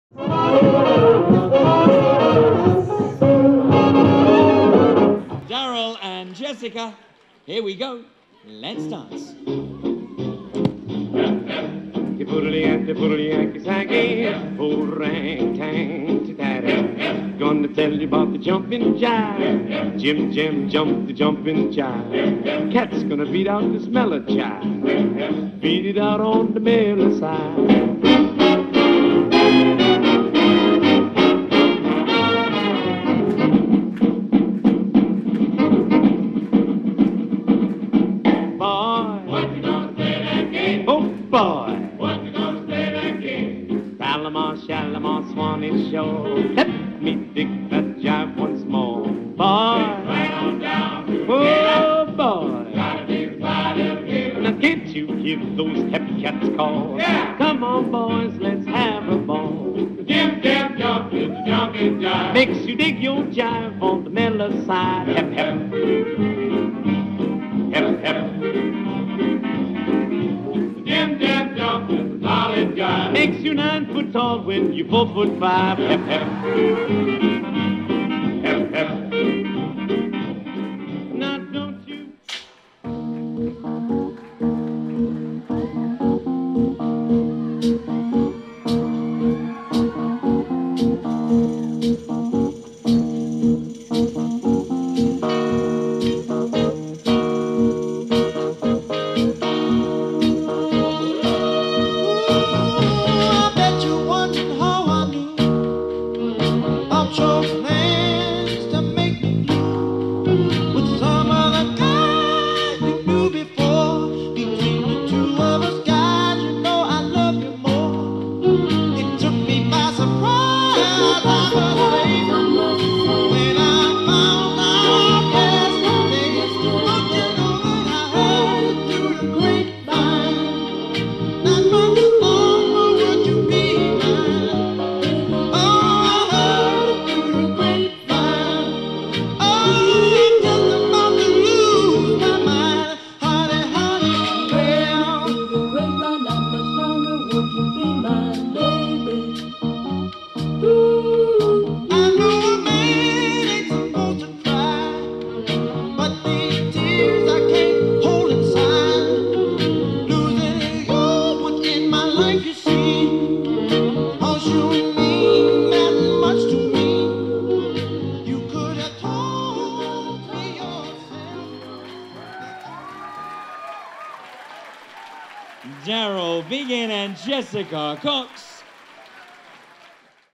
Boston Tea Party 2014, Daryl Begin & Jessica Cox, Invitational Crossover Jack & Jill
Invitational Crossover Jack & Jill